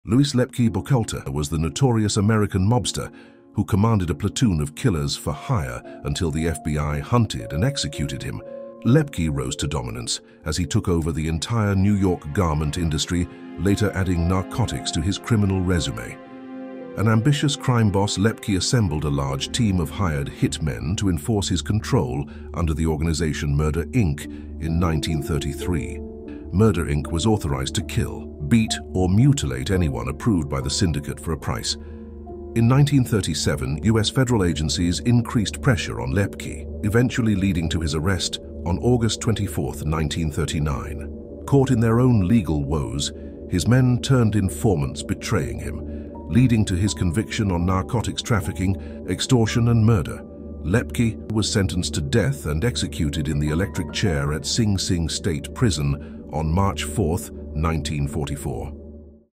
NOTORIOUS Mob Boss “Lepke” Of Murder Inc. Executed | March 4 1944
Louis Lepke Buchalter was the notorious American mobster who commanded a platoon of killers for hire until the FBI hunted and executed him.

Lepke rose to dominance as he took over the entire New York garment industry later adding narcotics to his criminal resume. An ambitious crime boss, Lepke assembled a large team of hired hit men to enforce his control under the organization Murder Inc. in 1933. Murder Inc. was authorized to kill, beat, or mutilate anyone approved by the syndicate for a price.

In 1937 U.S. federal agencies increased pressure on Lepke, eventually leading to his arrest on August 24th 1939. Caught in their own legal woes, his men turned informants betraying him, leading to his conviction on narcotics trafficking, extortion, and murder. Lepke was sentenced to death and executed in the electric chair at Sing Sing State Prison on March 4th, 1944.